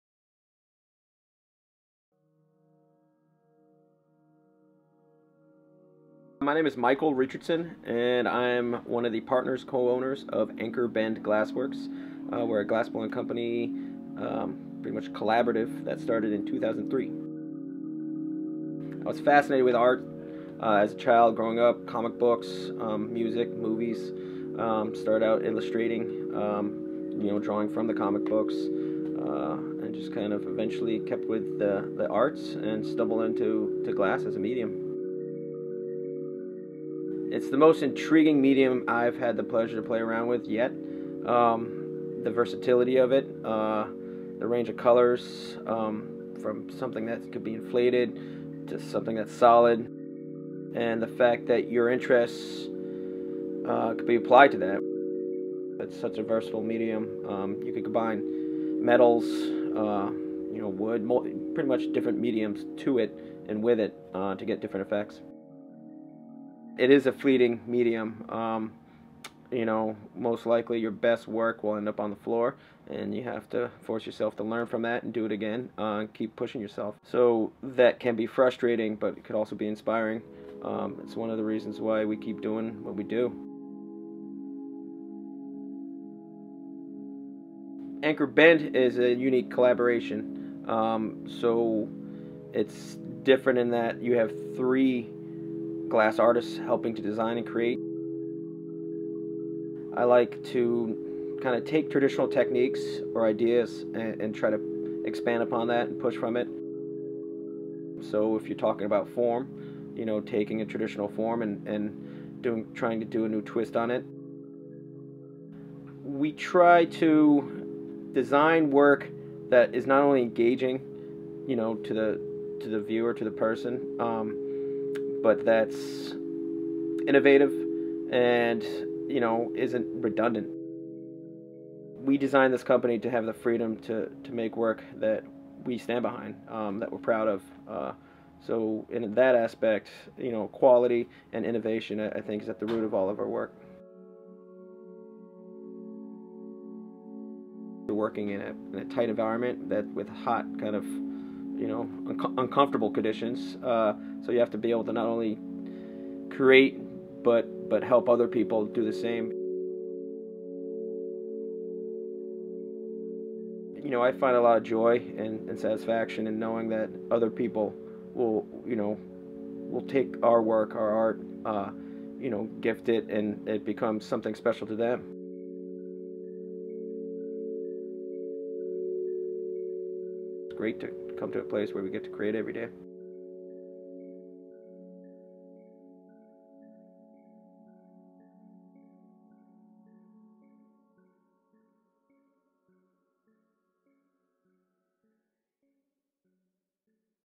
Glass Blowing: The Creation of Glass Art
Michael Richardson of Anchor Bend Glassworks discusses the creative process of working with glass.

Special thanks to:

Michael Richardson, Justin Tarducci, and Timothy Underwood of Anchor Bend Glassworks in North Kingstown, Rhode Island